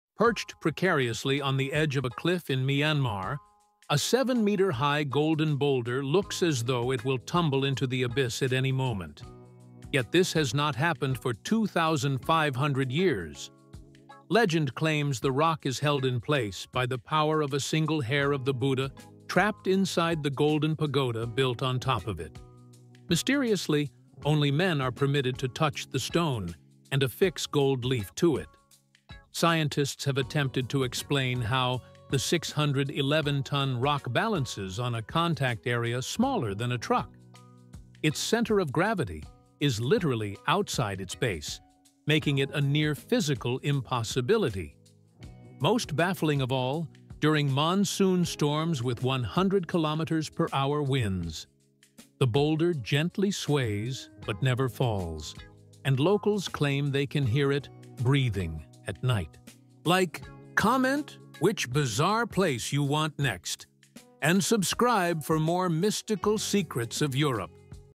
This Giant Rock Defies Gravity
Perched precariously on the edge of a cliff in Myanmar, the Kyaiktiyo Golden Rock is a 7-meter-high, 611-ton boulder that looks as though it will tumble into the abyss at any moment—yet this has not happened for 2,500 years.

Legend claims the rock is held in place by the power of a single hair of the Buddha trapped inside the golden pagoda built on its summit. Mysteriously, only men are permitted to touch the stone and affix gold leaf to it. Scientists are baffled: the rock's center of gravity is literally outside its base, balancing on a contact area smaller than a truck, a feat of near-physical impossibility. Most baffling of all, during monsoon storms with 100 km/h winds, the boulder gently sways but never falls, and locals claim they can hear it "breathing" at night.

Explore the science, the legend, and the mystery of the incredible Kyaiktiyo Pagoda!